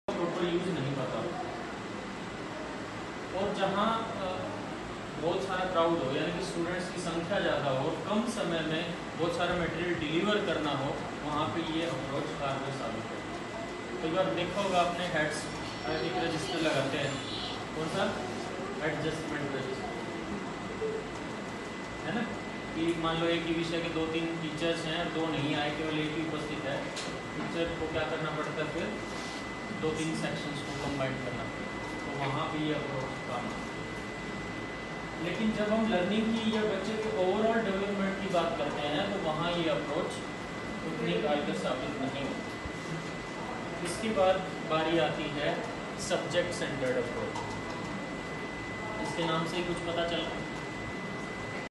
Approaches, methods of teaching learning&Counselling micro skills by Shri Deepak Lect in Psychology
5 days training of PGTs Biology at DIET Rewari